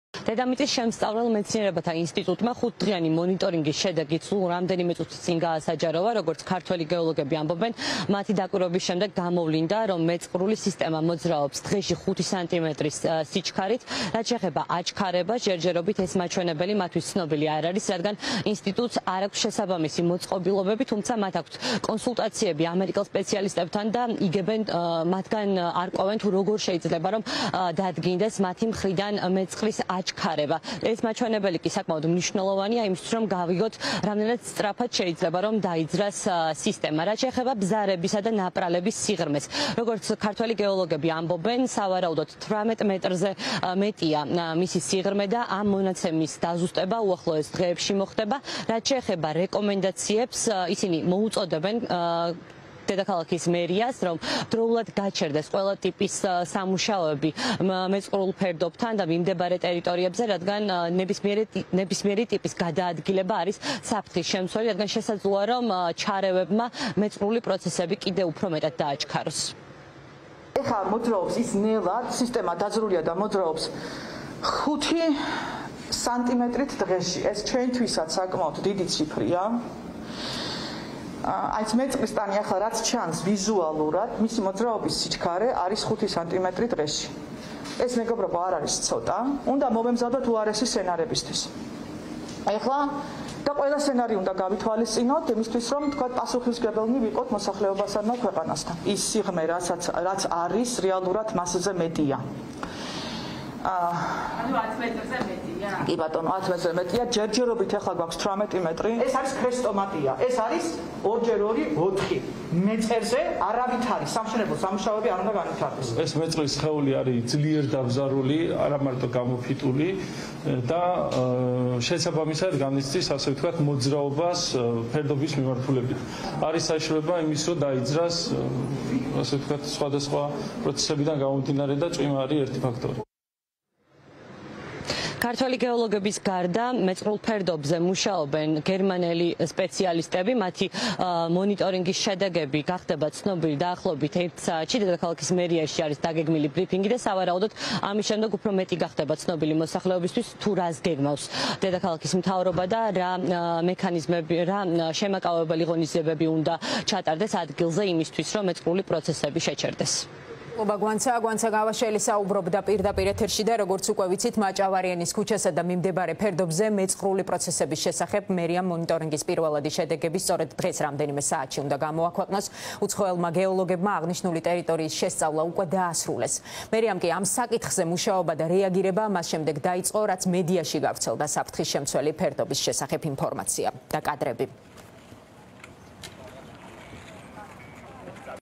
გეოლოგების დასკვნა
▪️ დედამიწის შემსწავლელ მეცნიერებათა ინსტიტუტის 5-დღიანი მონიტორინგის შედეგის მიხედვით, მეწყრული სისტემა ნუცუბიძე-ვაშლიჯვრის გზაზე დღეში 5 სანტიმეტრი სიჩქარით მოძრაობს